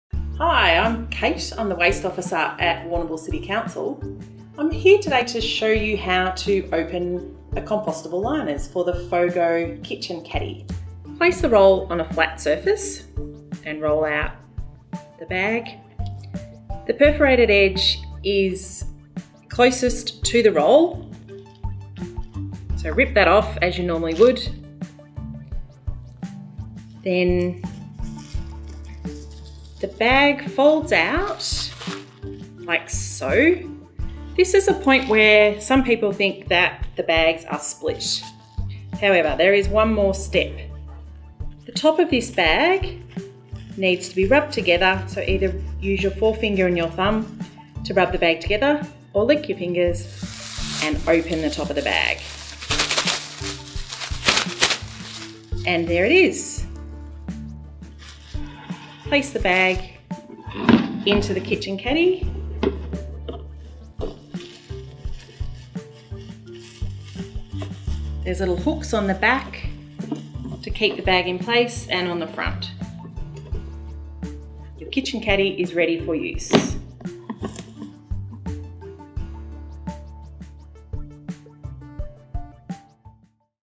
How to open your FOGO liners
The Food Organics Garden Organics (FOGO) collection service has now rolled out to all Warrnambool households, and so far, the vast majority of people are making great use of their new bin.

One of the most common issues we're hearing is people believing their bin liners are split when they aren't. So just to clarify, here's a short video showing how to open them.